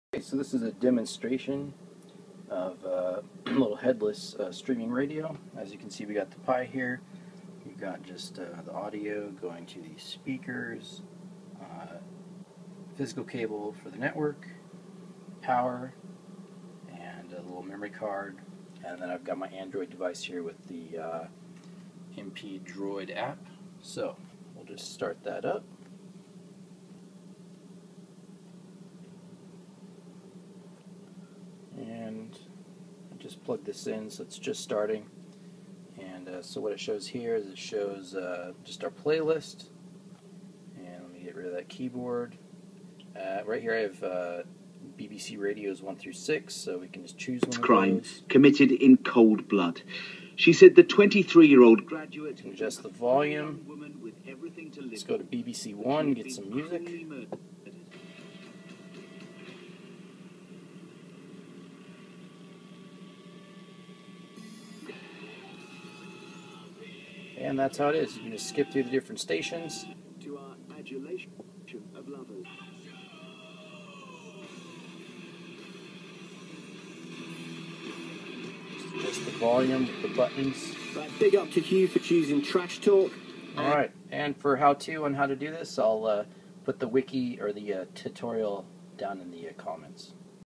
Raspberry PI - Headless Streaming Radio : MPC/MPD player
Demo of a headless streaming radio player. Instructions on how to setup your raspberry pi are located